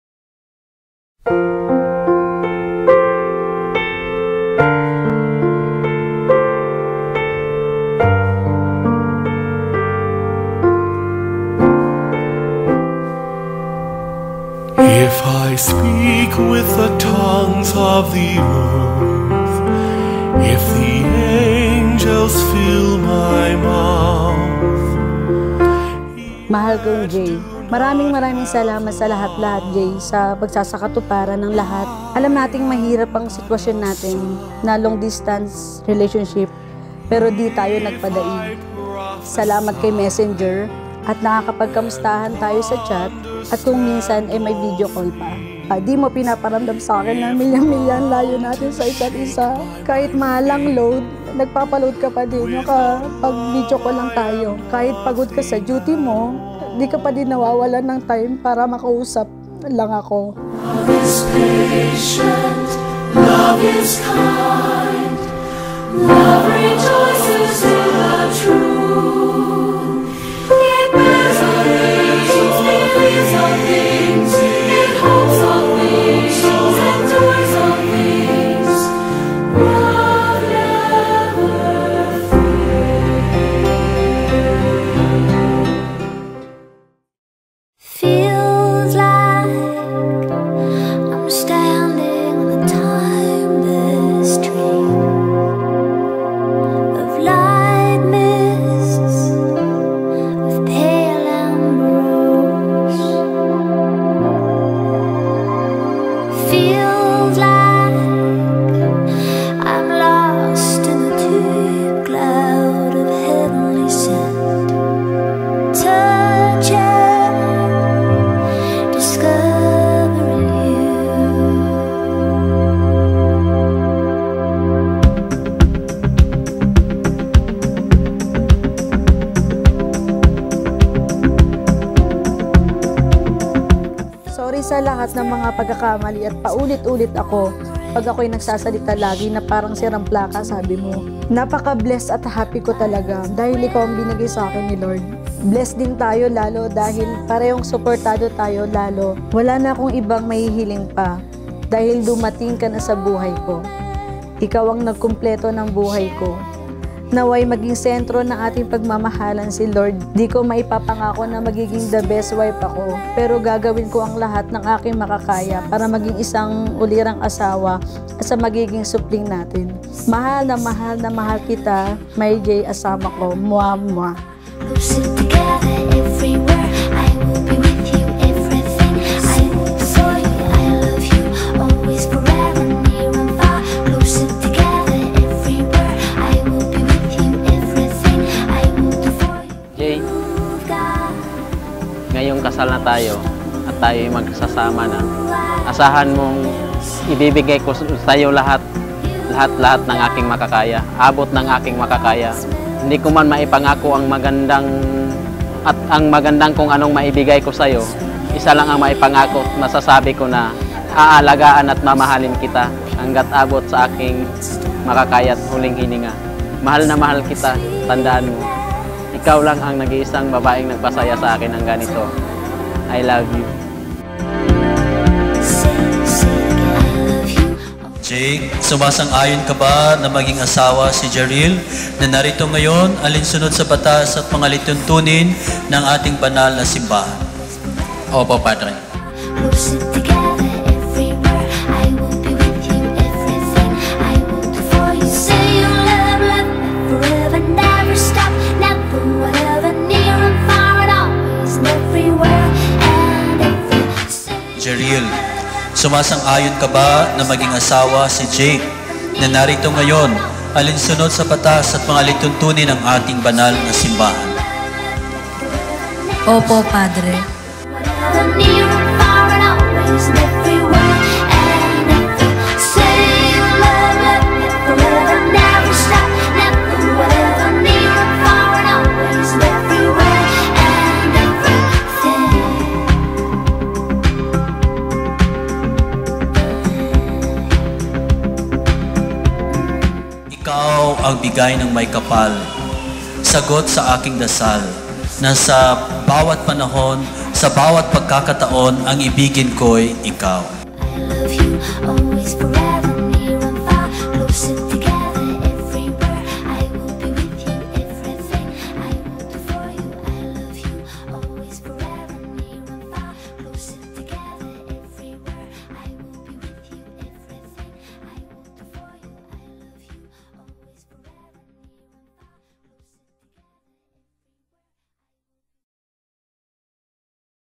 Jhearyl | Jake (Same Day Edit Video Highlights)
Jhearyl and Jake
(Same Day Edit Video Highlights)

P/V: NCM Productions

PhotoandVideoCoverage LiveStreaming Packages WITHIN YOUR BUDGET THIS PANDEMIC?

Because memories for a lifetime don't have to be so costly. 👍🥰